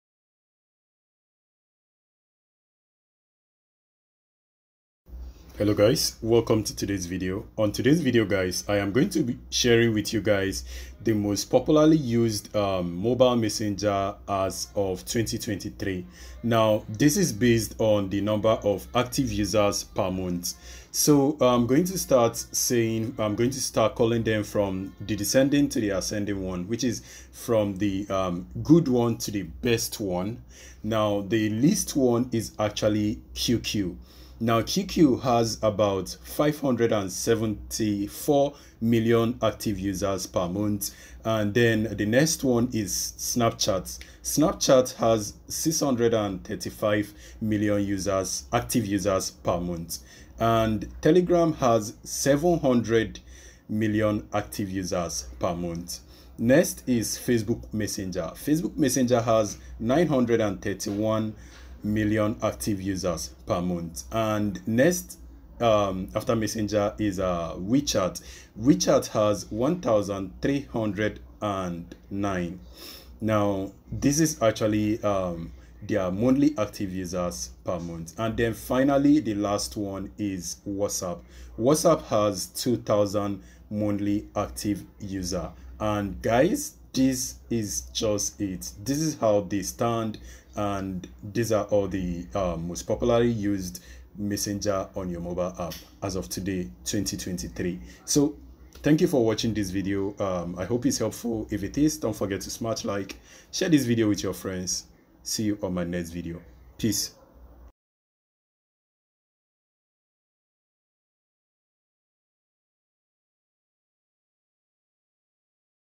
Most Popular Mobile Messenger 2023
In today's world, there are dozens of different mobile messenger apps available. But which ones are the most popular? In this video, we'll take a look at the top 5 most popular mobile messenger apps in 2023.

These are just a few of the many popular mobile messenger apps available. The best app for you will depend on your individual needs and preferences.